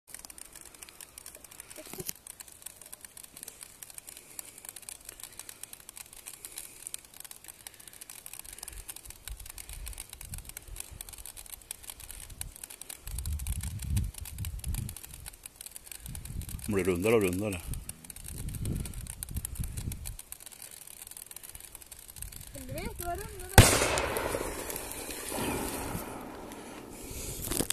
dryice
Dry ice exploading in a bottle.
Kolsyresnö som exploderar i en pet flaska.